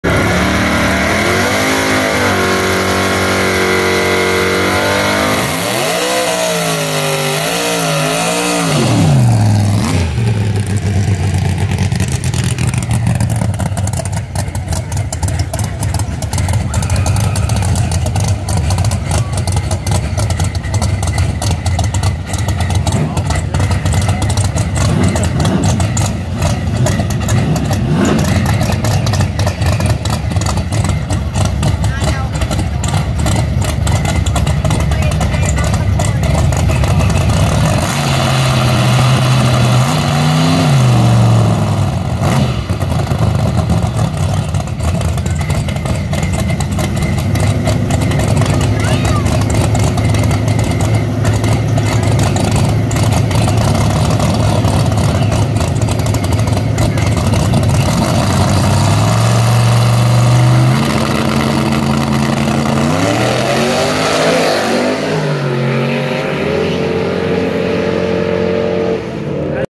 BullSnot Proud Drivers 3
Having a blast getting new customers at the Jackson Dragway, LLC ! It is amazing how many of them already know the herd of BullSnot products! BullSnot proud and grateful.